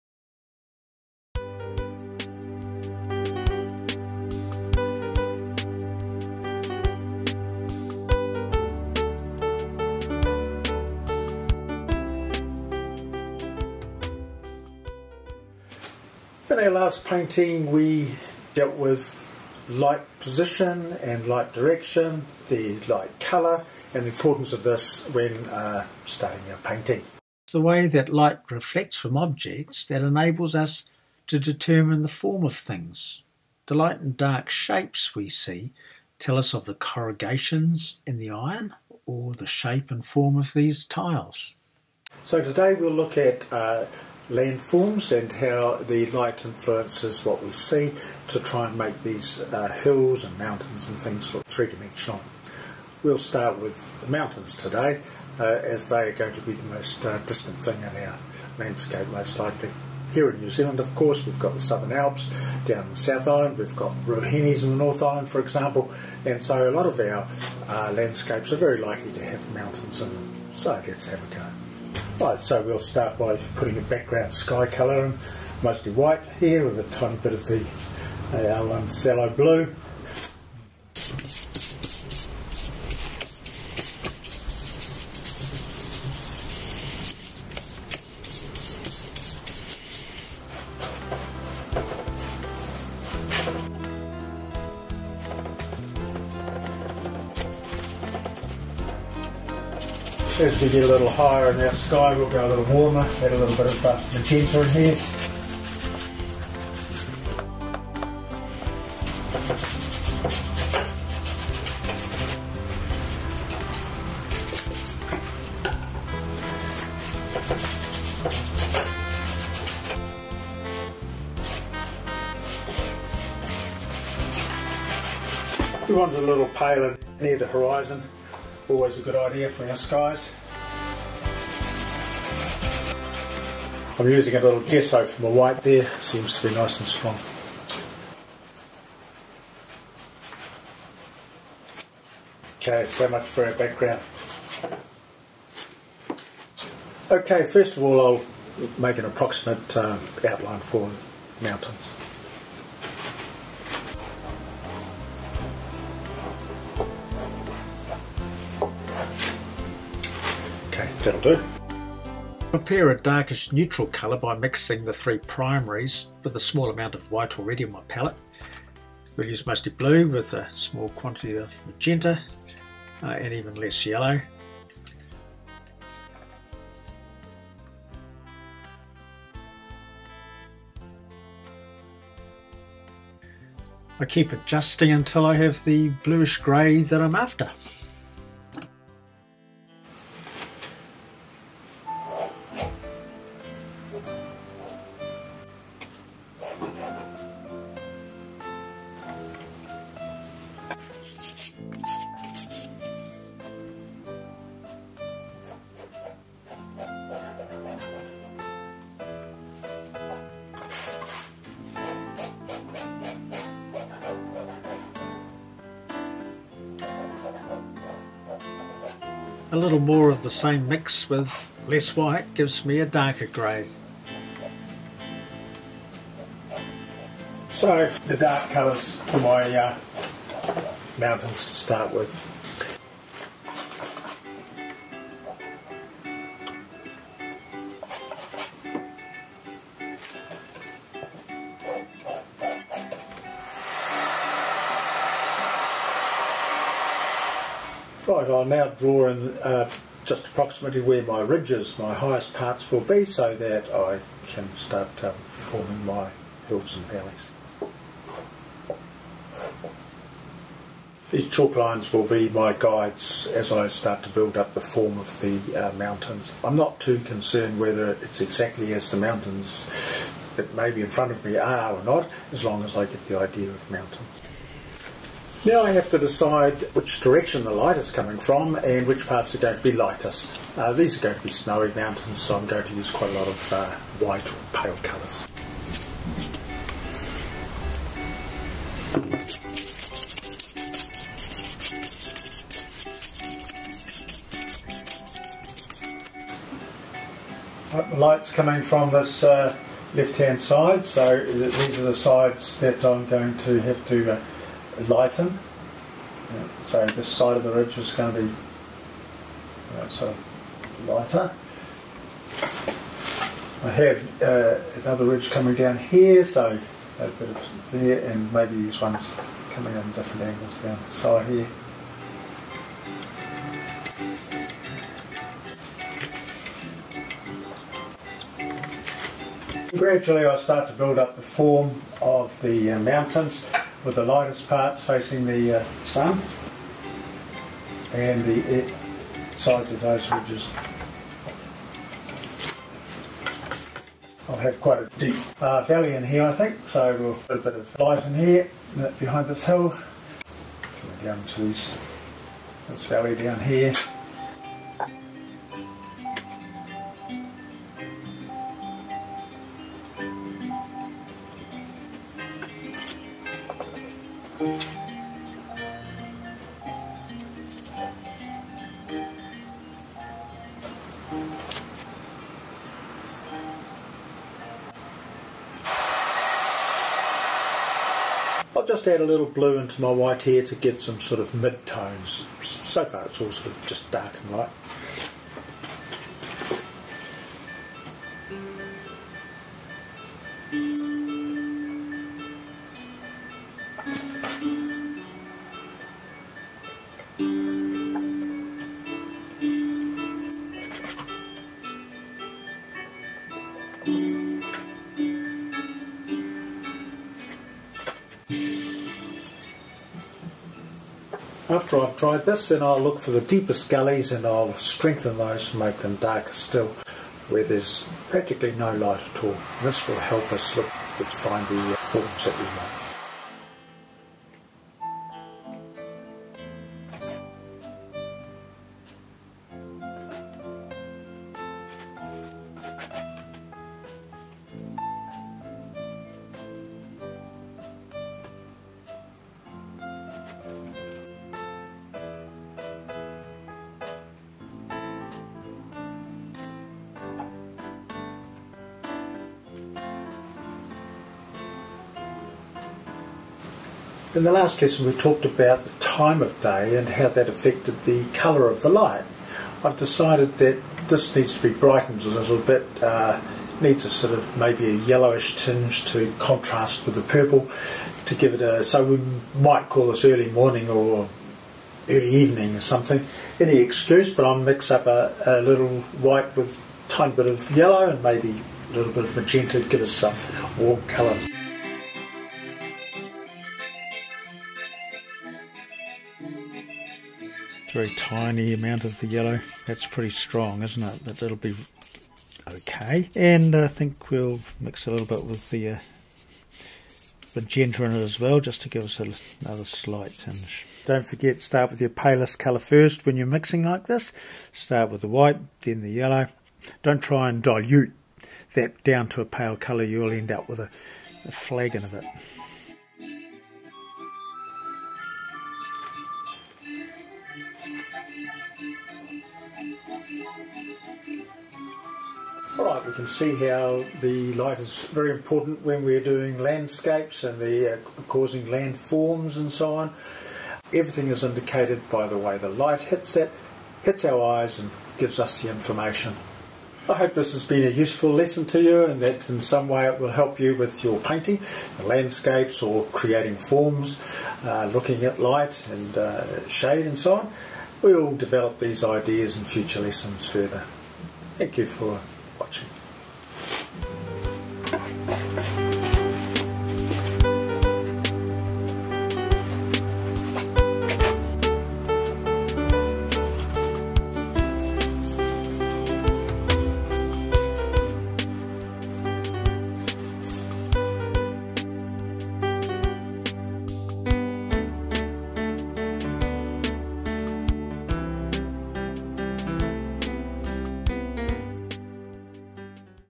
Easy way to Paint Mountains: Lesson 6 with Rob McGregor
This video is about the effect of light on landforms.